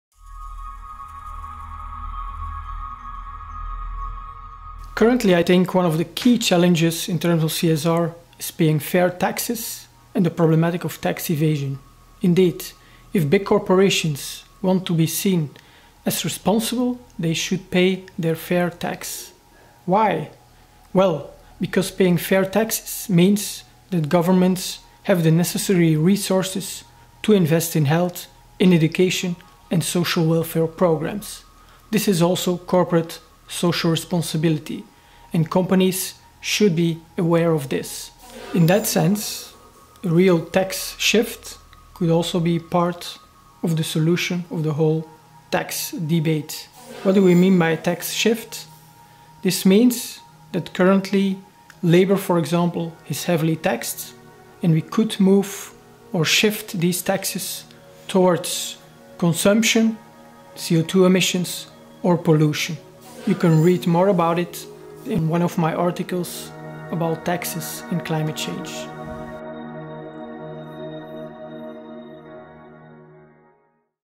Jordi Lesaffer Fair taxes | ENG master
Reflexions on Ethical Finances and Fair taxes by Jordi Lesaffer

Een film van | Un film de | A film by

Sophie Spillemaeckers
Ludovic Schweitzer